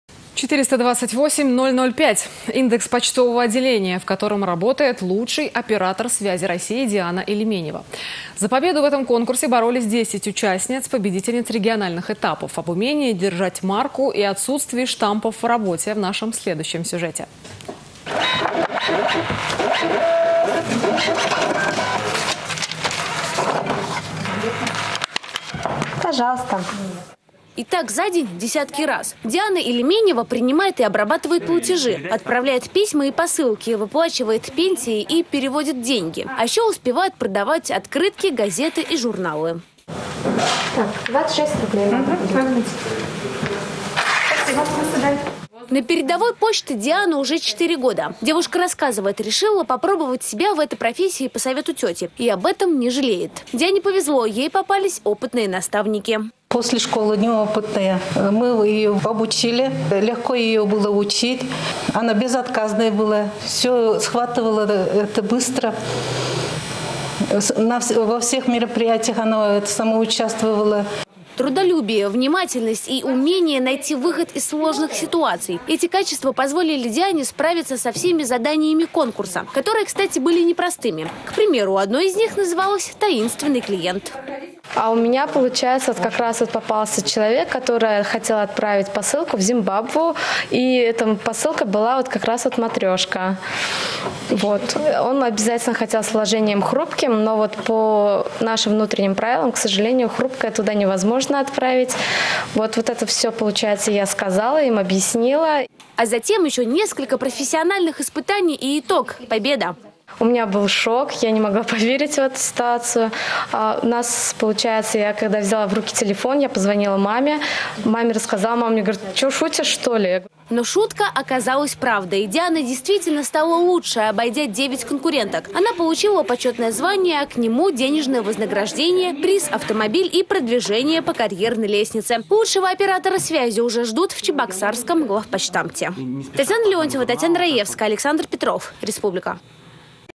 Лучший оператор почтовой связи
428005. Индекс почтового отделения, в котором работает лучший оператор связи России   Диана Илеменева. За победу в этом конкурсе боролись 10 участниц — победительниц региональных этапов. Об умении держать марку и отсутствии штампов в работе- в нашем следующем сюжете